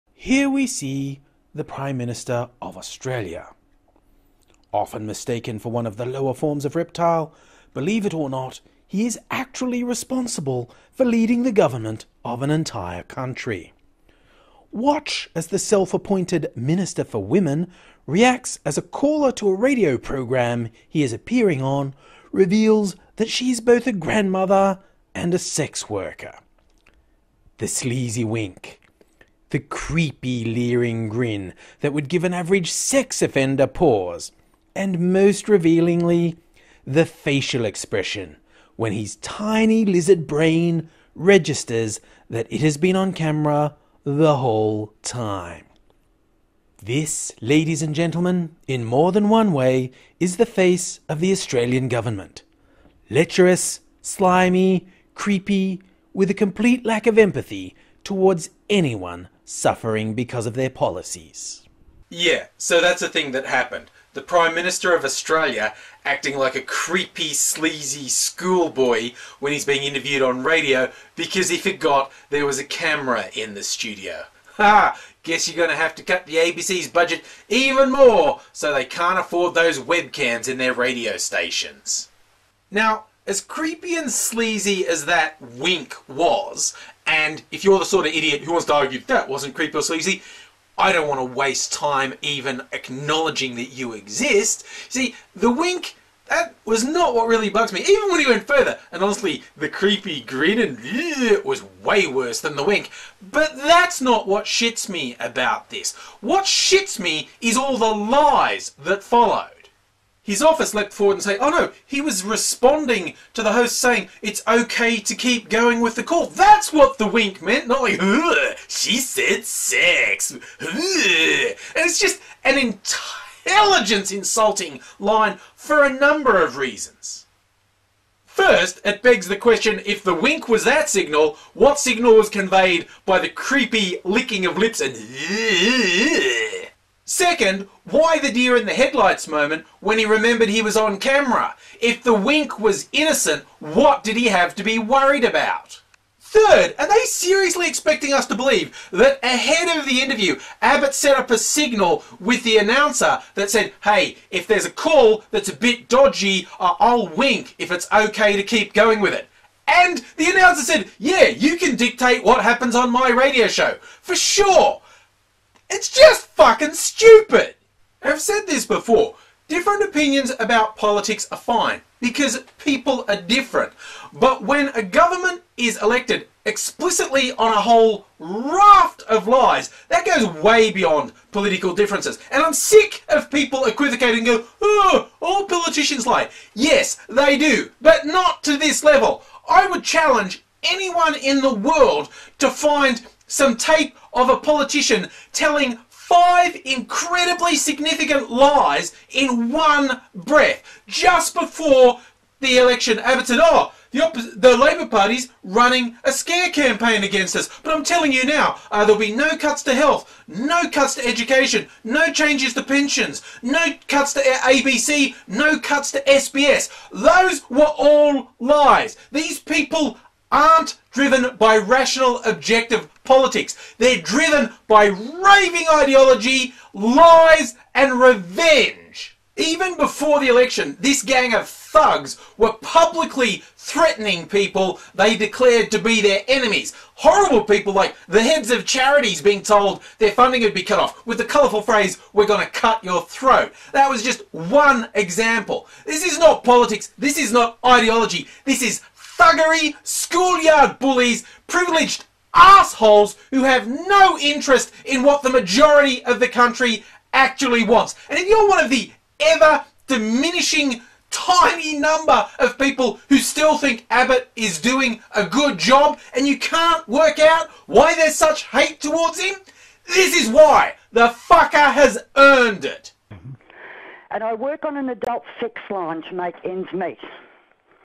Tony Abbott is a Winker
Learn what it has been like for the granny who works on a phone sex line to become a social media senstation:

There are really cheap shirts here that's still pretty good quality (and you can customise them!)